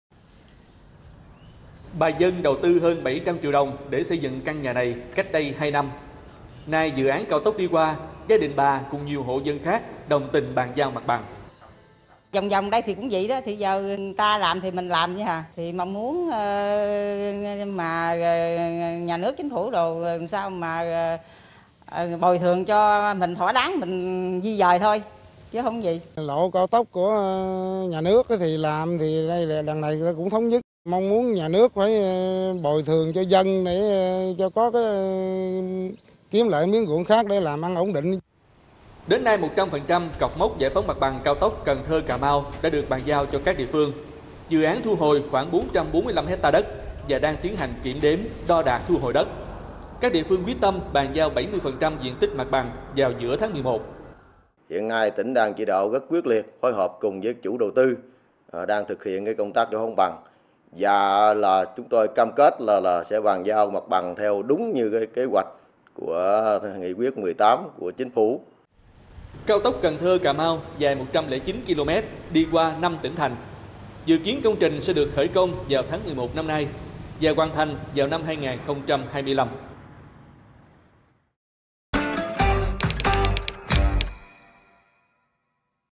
KHẨN TRƯƠNG GIẢI PHÓNG MẶT BẰNG CAO TỐC CẦN THƠ - CÀ MAU | VTV5
Cần Thơ - Cà Mau là đoạn cao tốc cuối cùng nằm trên tuyến cao tốc Bắc - Nam. Dự án thu hồi khoảng 445ha đất. Hiện công tác giải phóng mặt bằng đang được các tỉnh, thành thực hiện khẩn trương để chuẩn bị  khởi công.